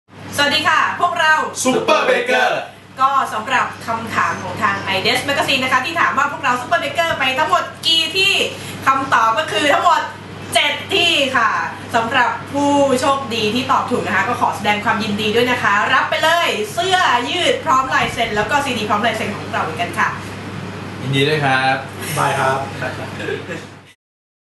ร่วมกิจกรรม กับ iDes+ และ superbaker
เล่นเกมส์ชิงรางวัล CD และ T-Shirt ของ superbaker พร้อมสายเซนต์ จากคำถาม superbaker พาเที่ยว iDes+ Pattaya